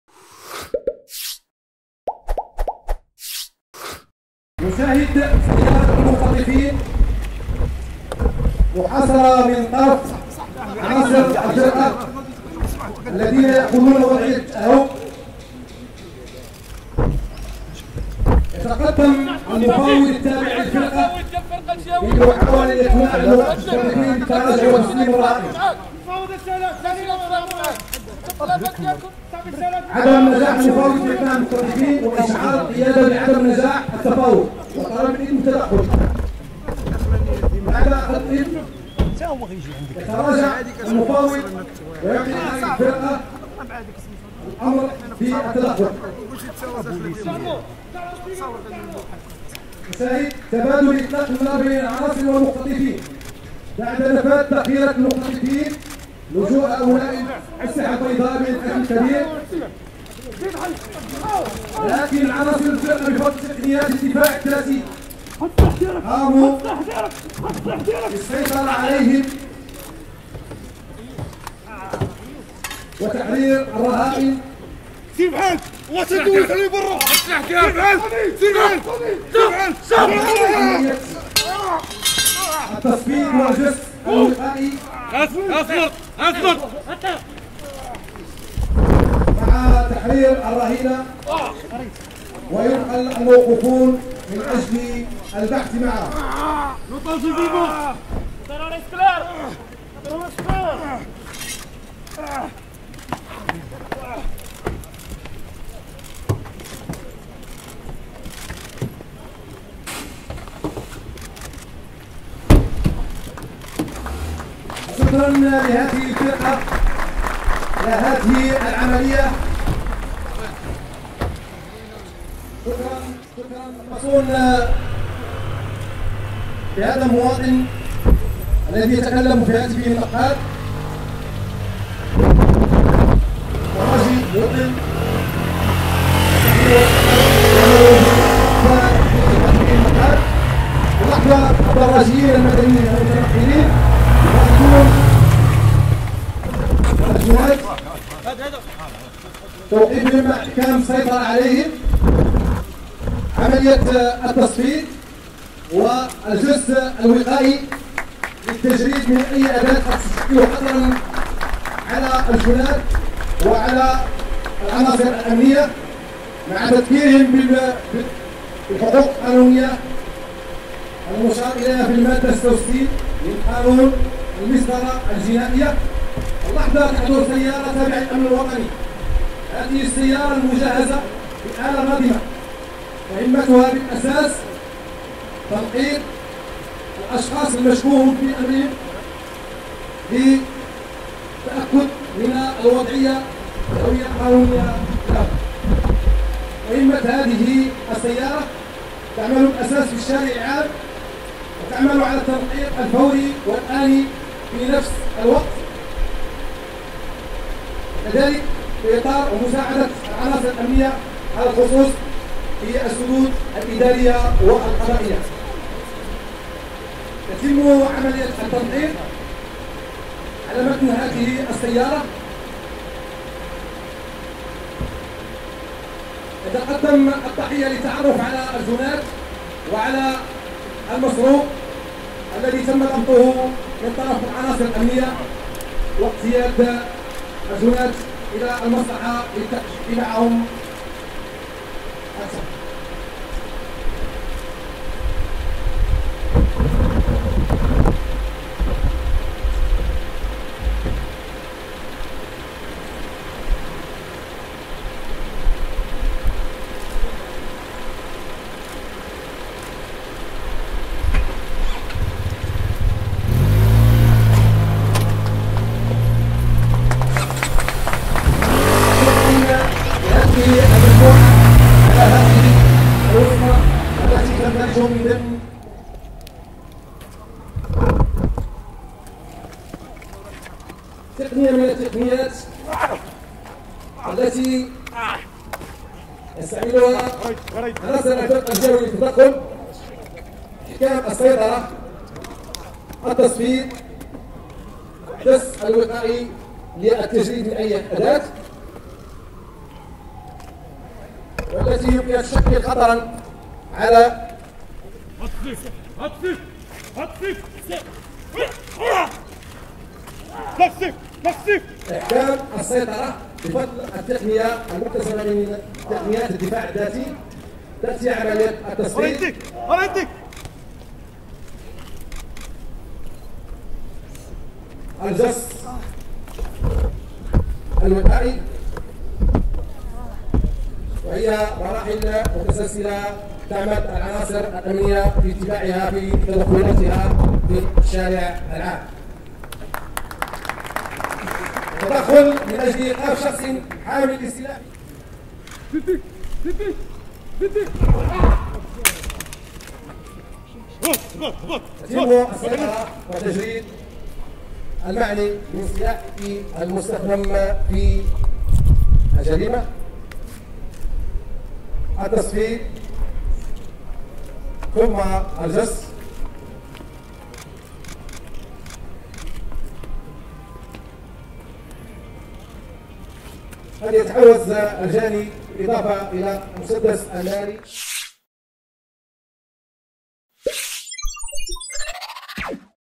إستعراض رجال الحموشي.. مهارات فنية وتدخلات احترافية عالية بمناسبة الذكرى 61
نورسعيد كمال: الوكالة الوطنية للنشر والصور الصحفية  ANP PRESS
للمزيد من الأخبار زوروا موقعنا الإلكتروني